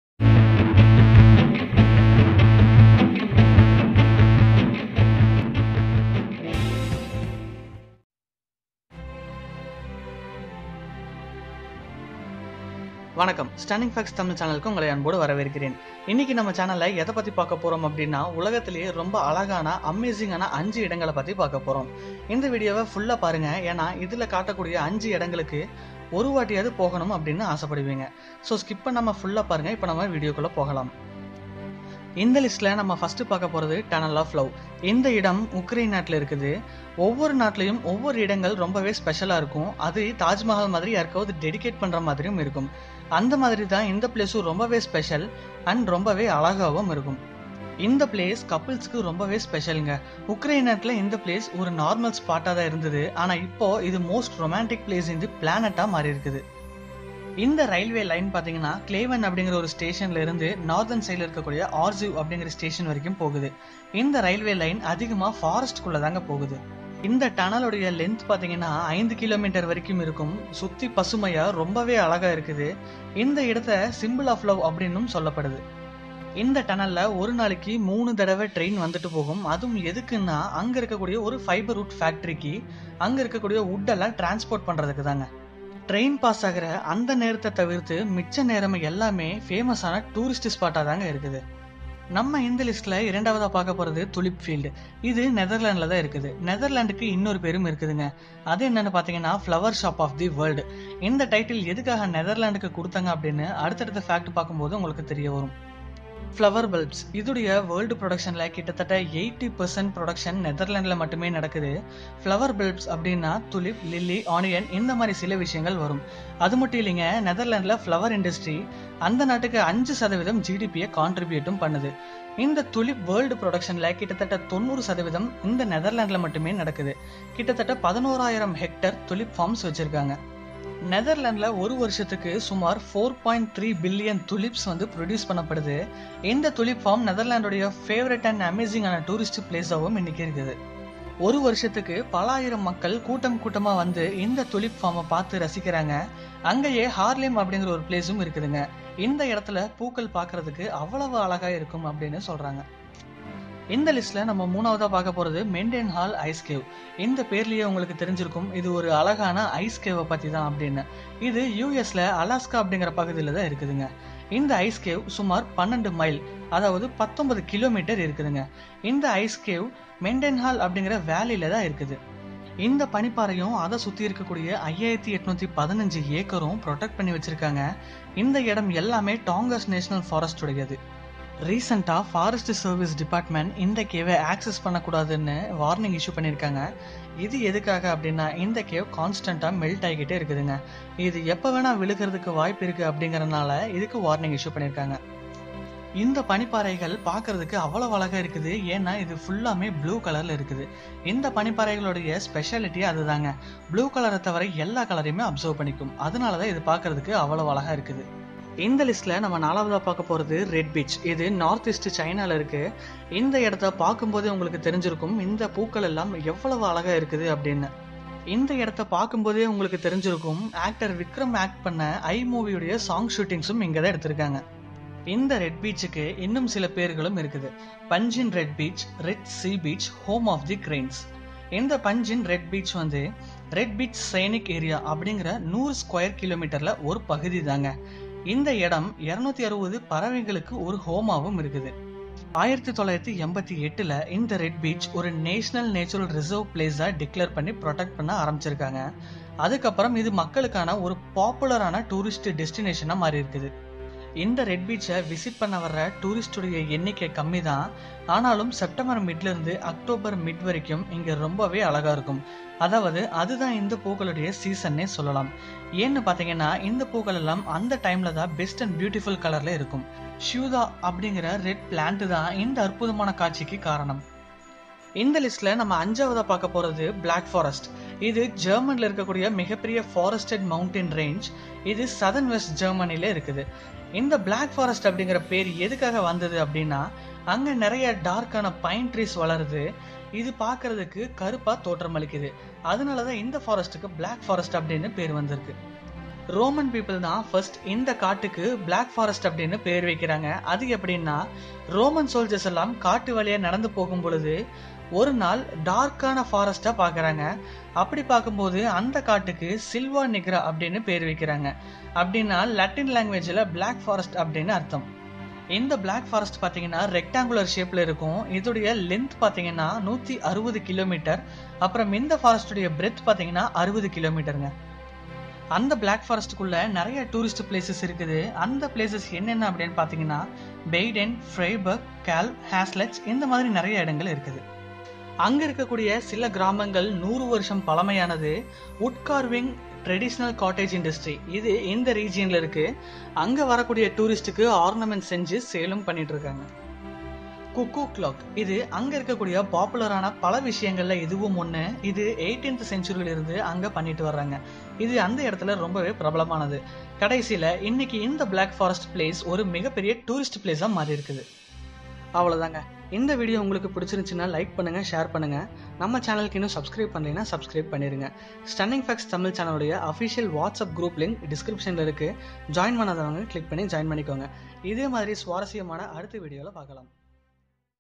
Most Romantic place in the Planet | Tamil | Stunning Facts Tamil
This video explains 5 Amazing Places in the world

Content, Edit & Voice Over

Nikkil Viswanath
Kathirvel Murugesan

Videvo. com